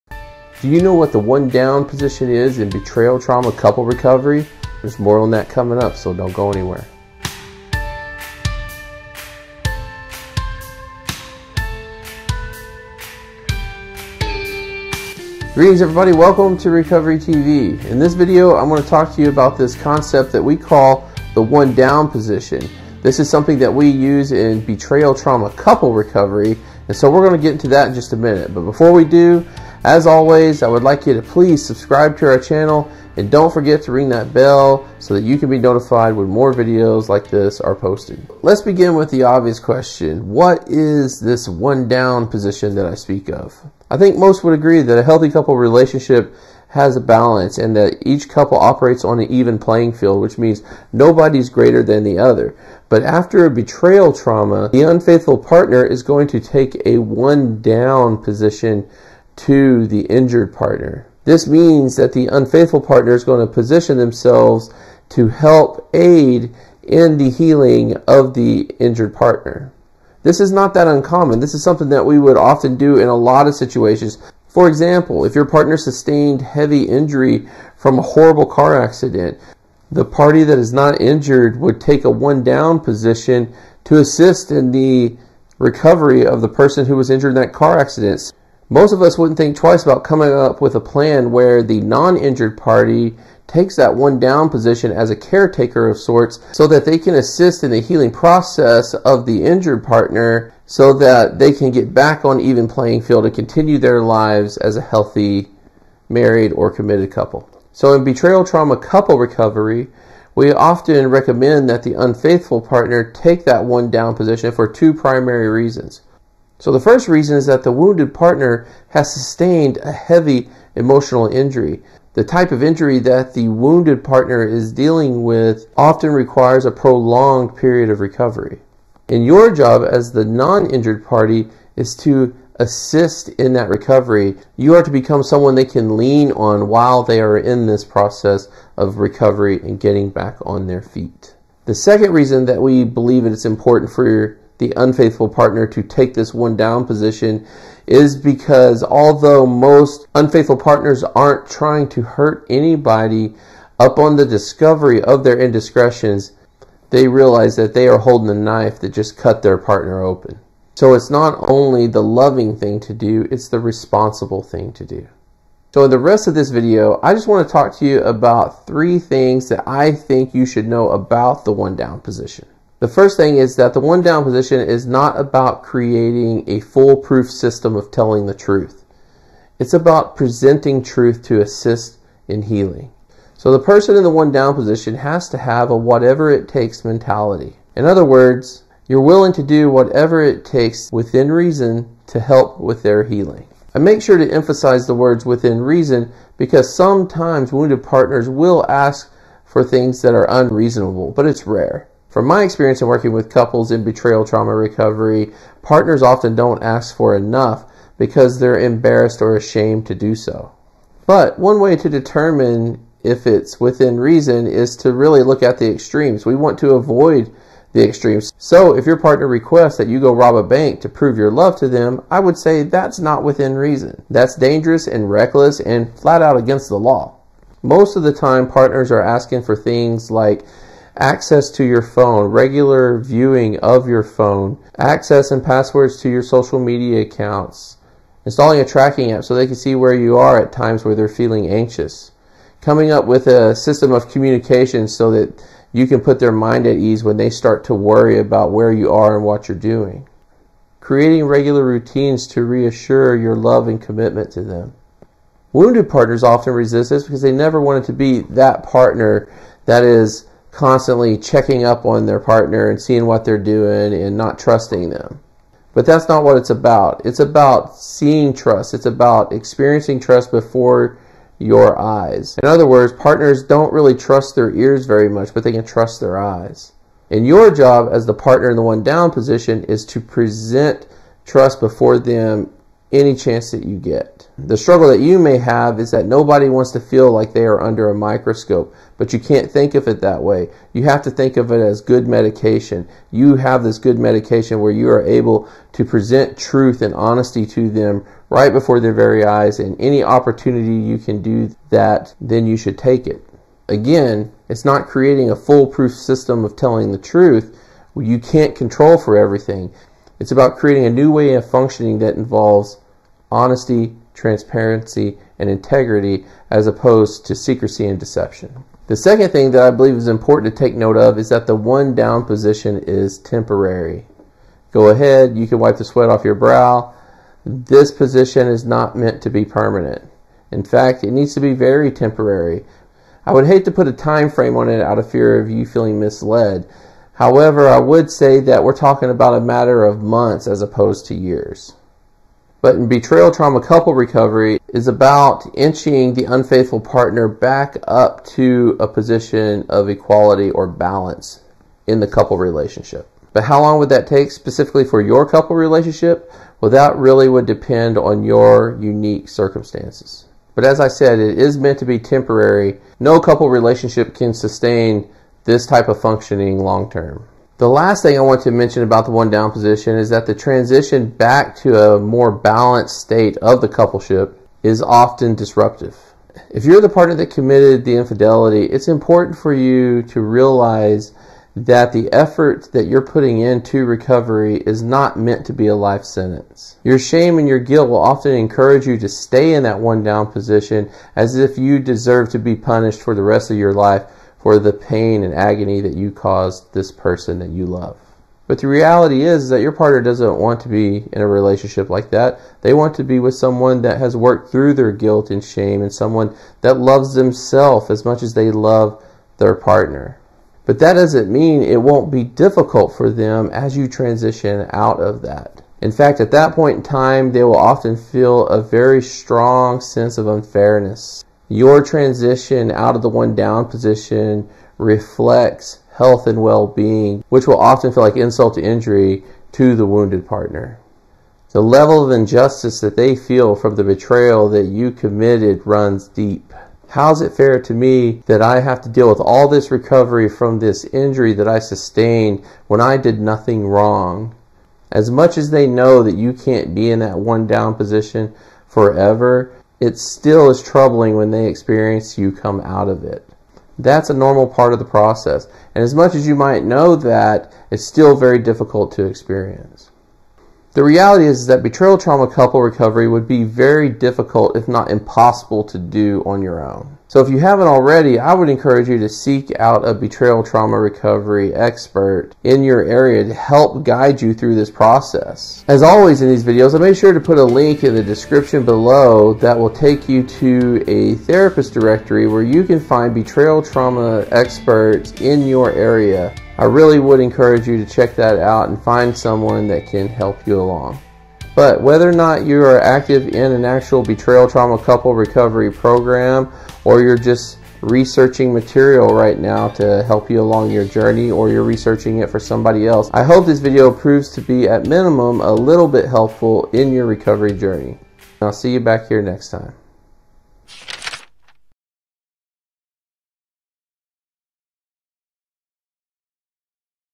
The One-Down Position in Betrayal Trauma Couple Recovery
Have you ever heard of the one-down position in betrayal trauma couple recovery or affair recovery? If you are part of a marriage or couple relationship that has been impacted by infidelity or adultery or even sex addiction, then you need to watch this video.

The discovery of infidelity in a relationship is often devastating to the relationship.  It creates an emotional wound that is painful in ways hard to put into words.  Since it is not a physical injury, couples will often try to proceed in life as normal because on the surface, there appears to be no injury.  But, deep inside, there is a gaping wound that needs to be tended to right away.

This is where the one-down position comes in.  It is a position of caretaking by the unfaithful or acting out partner.  Not only is it a loving act, but it also the responsible thing to do given that the injury was created by the betrayal of the unfaithful partner.

In this video, licensed therapist and betrayal trauma expert, Joshua Nichols, discusses the importance of one down position as well as three important facets of the one down position. Hope it is helpful.